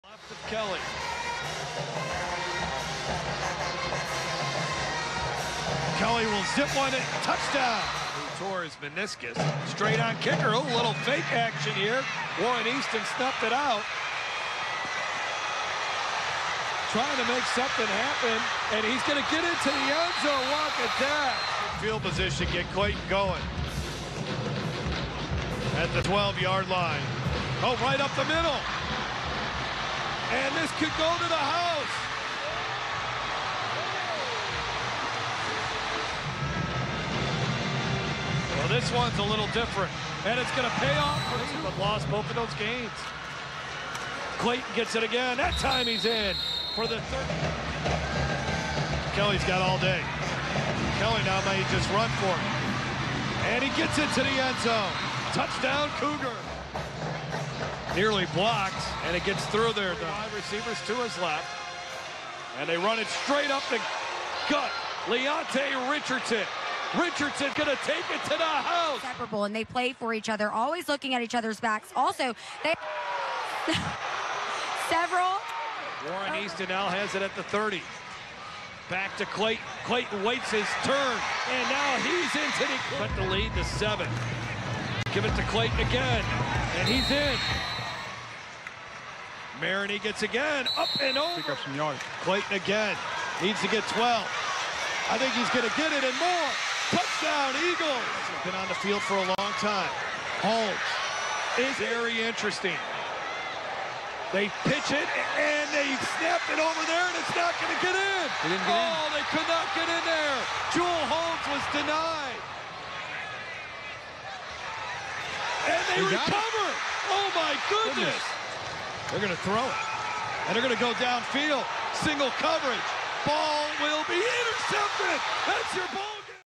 Edna Karr vs Warren Easton 2019
Edna Karr 35 vs Warren Easton 34
Class AAAA State Championship Highlights
LHSAA 2019